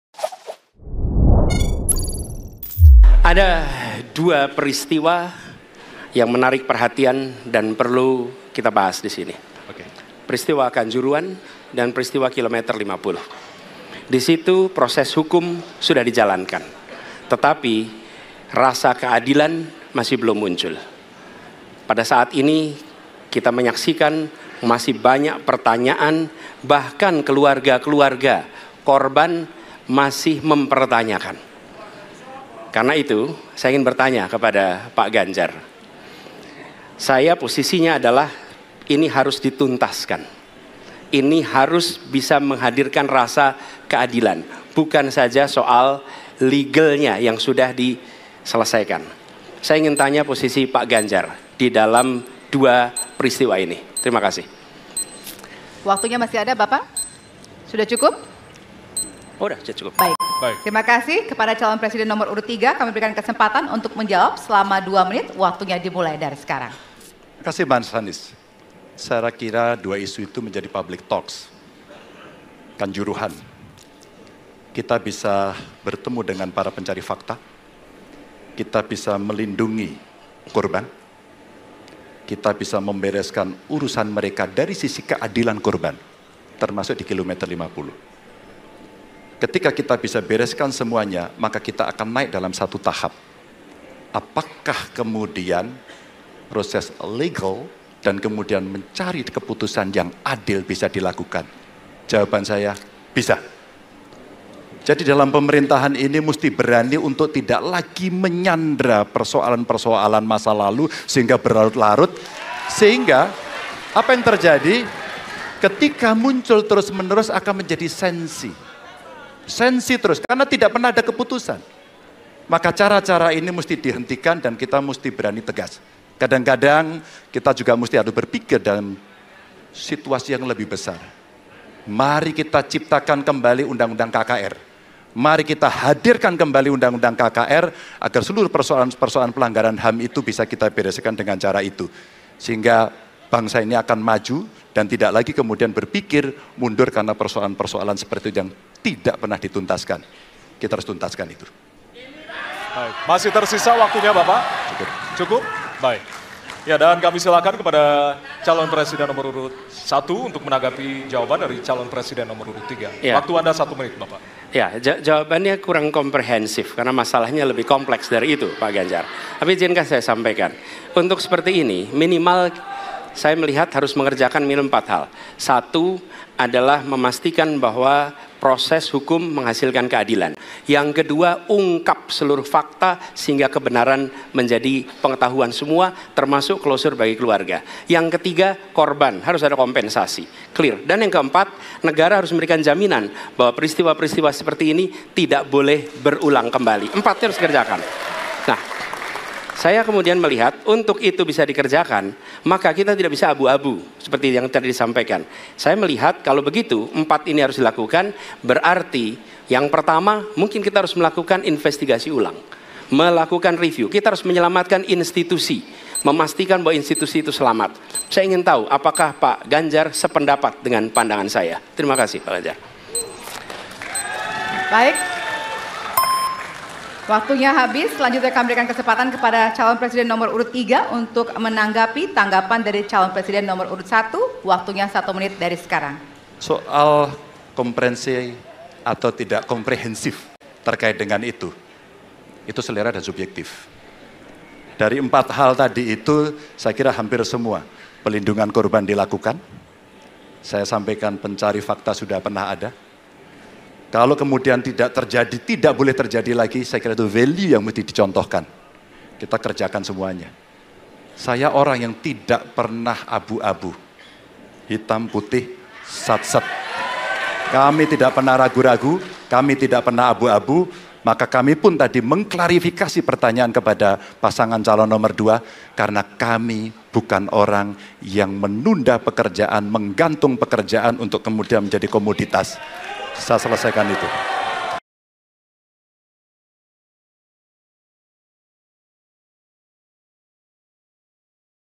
Langkah Anies-Ganjar Agar Kasus Kanjuruhan-KM 50 Tak Terulang
Capres nomor urut 1 Anies Baswedan bertanya soal sikap Capres nomor urut 3 Ganjar Pranowo terkait kasus Kanjuruhan dan KM 50. Baik Anies dan Ganjar pun sama-sama memberikan langkah yang dilakukan agar kejadian serupa tak terulang.